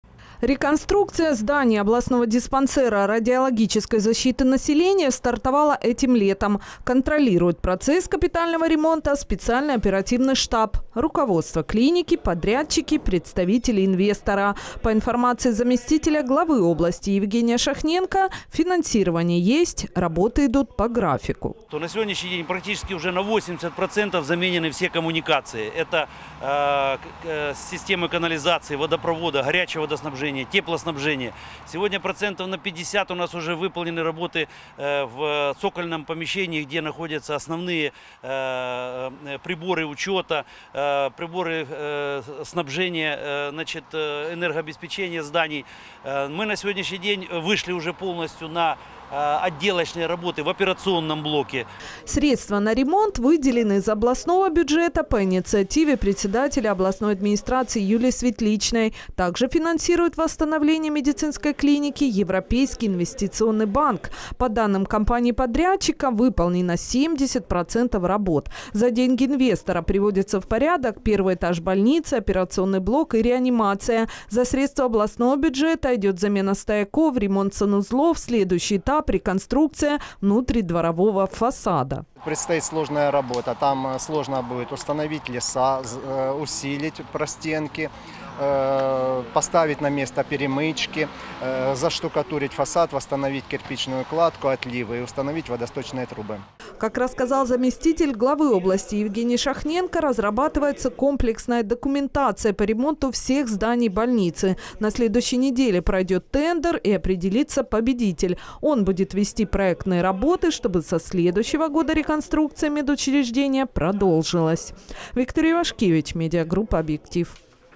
В областной больнице для чернобыльцев продолжается ремонт
В больнице для ликвидаторов аварии на ЧАЭС выполнены 70% ремонтных работ. Завершается замена коммуникаций, реконструкция операционного блока и реанимации. Средства на ремонт клиники выделены из областного бюджета.